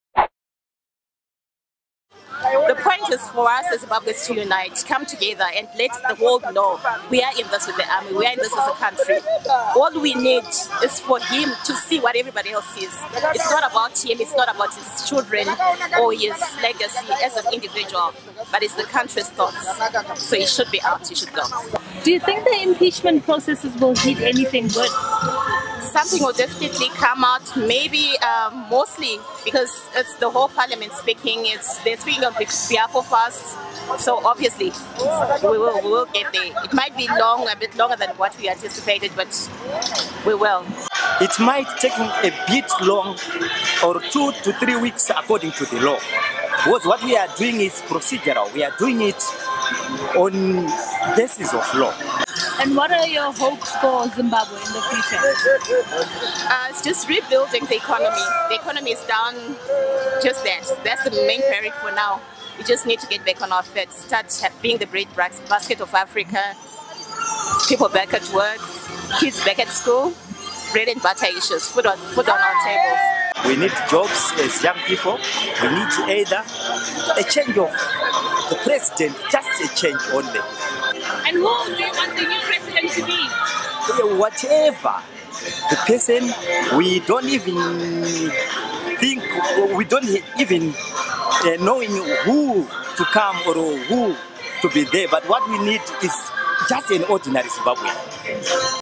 "Ons soek net 'n gewone Zimbabwiër"
Betogers by Africa Unity Square in Harare sê hul sal geduldig wag vir die prosesse wat moet volg om van Mugabe ontslae te raak. Hul soen net 'n gewone Zimababwiër as hul nuwe leier.